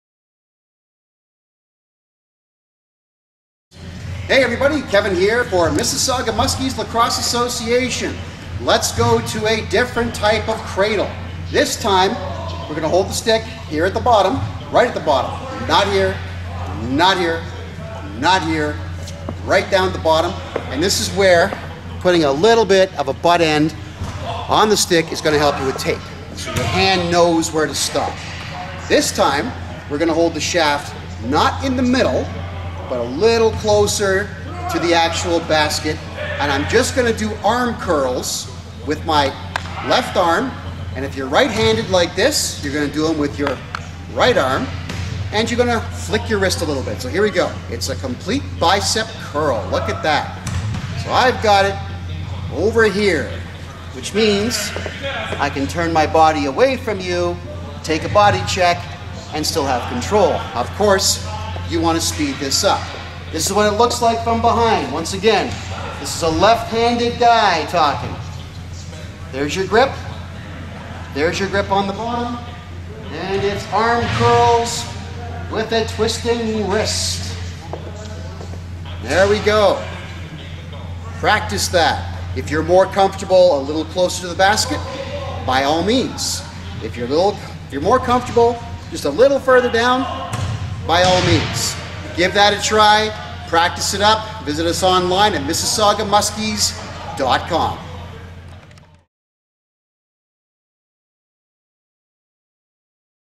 How To Cradle The Ball In Lacrosse Lesson 2 Mississauga Muskies Lacrosse Association OLA
This is the bicep curl style of cradling the ball. These are lessons in How to cradle the ball in Lacrosse with Kevin J. Johnston in Mississauga Lacrosse.

The Mississauga Muskies are a Lacrosse Academy and House League.